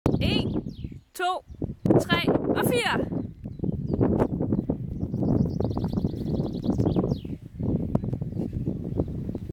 24 m løb ca.
Jeg har ret god acceleration og meget sprintkraft.